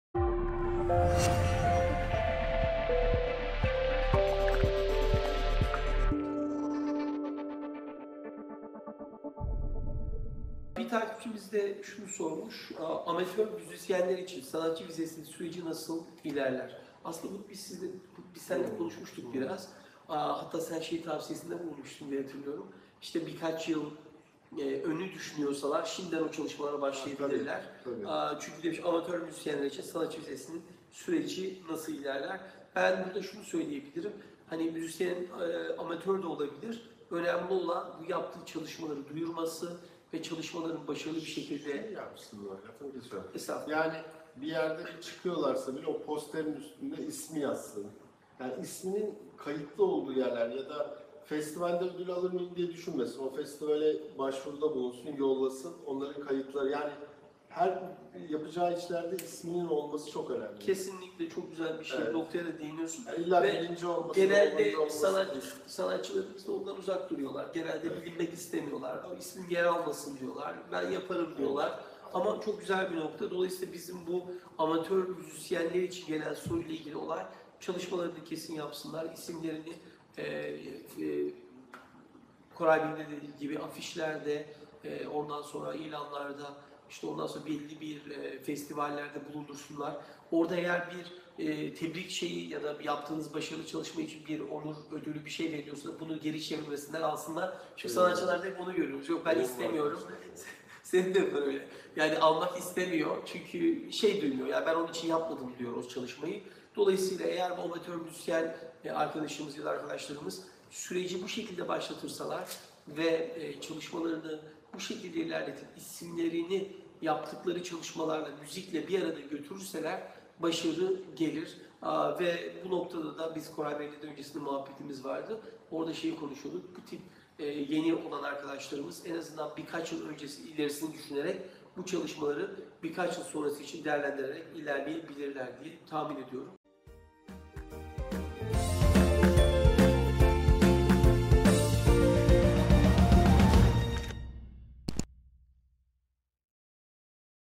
AMATÖR MÜZİSYENLER İÇİN SANATÇI VİZESİ SÜRECİ NASIL İLERLER ?
Soru: Amatör müzisyenler için sanatçı vizesinin süreci nasıl ilerler ?

Avukat Ayhan Ogmen'in, 3 Haziran 2018 Pazar günü, Facebook - Instagram - YouTube - Periscope Ortak Canlı Yayınında; Dünyaca Ünlü Hollywood Fotoğraf Sanatçısı Koray Kasap İle O1 Sanatçı Vizeleri Özel Programı.

PROGRAM KONU BAŞLIKLARI:

BAŞARILI FOTOĞRAF ÇEKİMİNİN PÜF NOKTALARI
İSTANBUL’DAN HOLLYWOOD’A UZANAN BİR BAŞARI HİKAYESİ
AMERİKA’DA FOTOĞRAF SANATÇISI OLMANIN YOLLARI

O-1 SANATÇI VİZESİ NEDİR ?
O-1 SANATÇI VİZESİ NASIL ALINIR ?
O-1 SANATÇI VİZESİ ŞARTLARI NELERDİR ?
O-1 SANATÇI VİZESİNİ KİMLER ALABİLİR ?